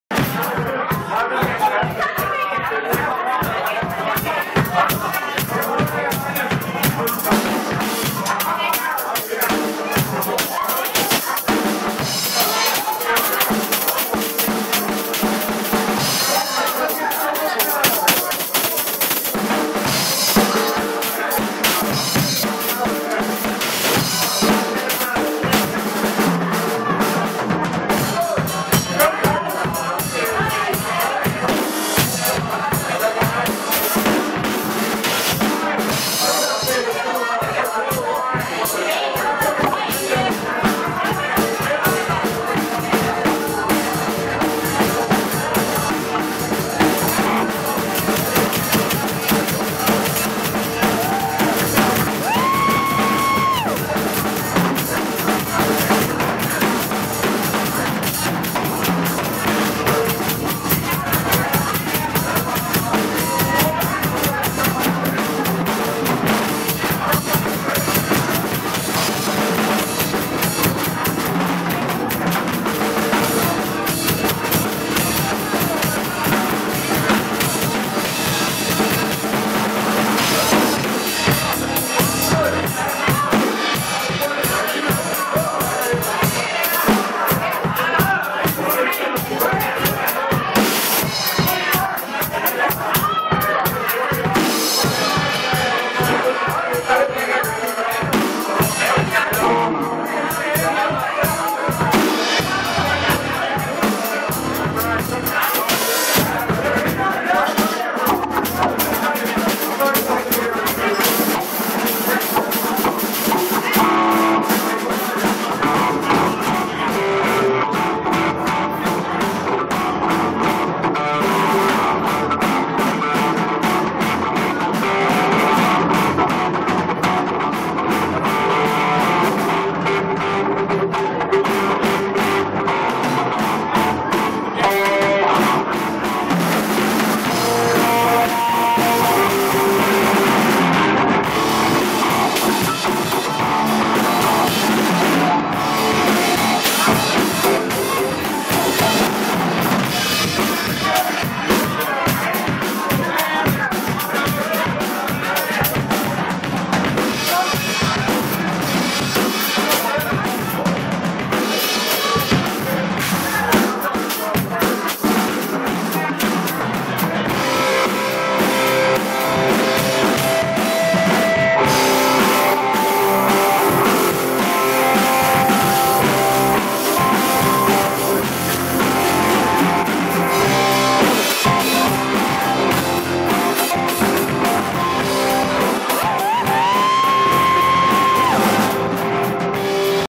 Jonny Waters & Company Jam Sesh Pt. 2
Jonny Waters & Company Jam Sesh at Basnight's Lone Cedar Cafe - 2/23/13